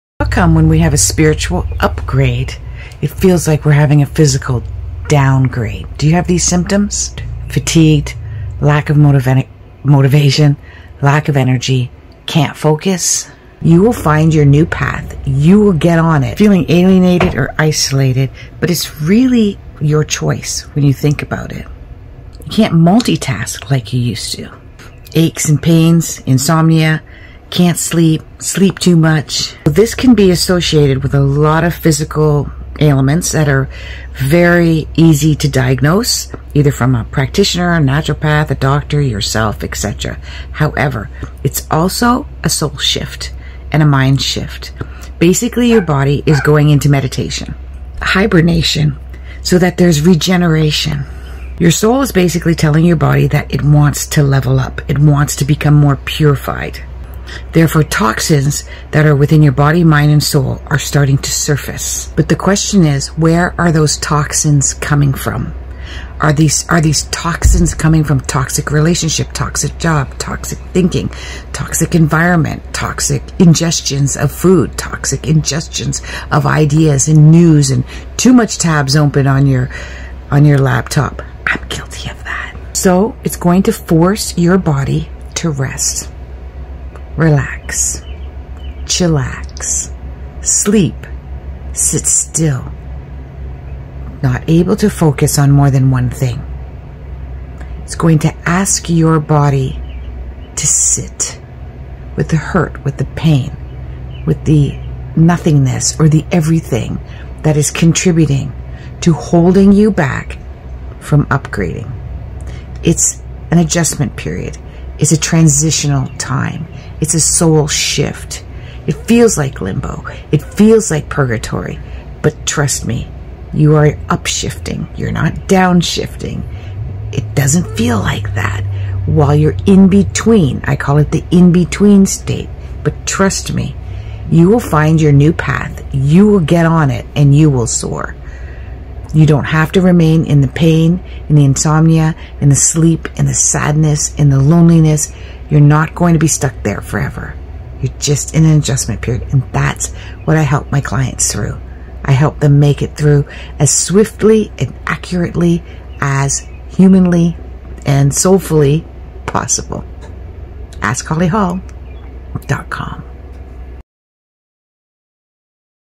WHY A SPIRITUAL UPGRADE FEELS LIKE A PHYSICAL DOWNGRADE ?
And its really a superpower!
How come when we have a spiritual upgrade it feels like we're having a physical downgrade do you have these symptoms fatigued lack of motiva motivation lack of energy can't focus you will find your new path you will get on it feeling alienated or isolated but it's really your choice when you think about it you can't multitask like you used to aches and pains insomnia can't sleep sleep too much this can be associated with a lot of physical elements that are very easy to diagnose either from a practitioner naturopath a doctor yourself Etc
However it's also a soul shift and a mind shift basically your body is going into meditation hibernation so that there's regeneration your soul is basically telling your body that it wants to level up
It wants to become more purified therefore toxins that are within your body and soul are starting to surface  The question is where are those toxins coming from are these are these toxins coming from toxic relationship toxic job toxic thinking toxic environment toxic ingestions of food toxic ingestions of ideas and news and too much tabs open on your on your laptop; I'm guilty of that!
So it's going to force your body to rest relax chillax sleep sit still not able to focus on more than one thing it's going to ask your body to sit with the hurt with the pain with the nothingness or the everything that is contributing to holding you back from upgrading it's an adjustment period, it's a transitional time, it's a soul shift!
It feels like limbo it feels like Purgatory but trust me you are upshifting you're not downshifting it doesn't feel like that while you're in between I call it the in-between state.
But trust me you will find your new path you will get on it and you will soar you don't have to remain in the pain in the insomnia in the sleep in the sadness in the loneliness you're not going to be stuck there forever you're just in an adjustment period
And that's what I help my clients through I help them make it through as swiftly and accurately as humanly and soulfully possible!

Holly Hall
Clairvoyant Life Coach

LINKT.EE/Askhollyhall HERE ALL MY LINKS ARE HERE!
MY BOOK- AMAZON 101 Answers from the Universe

FIND ME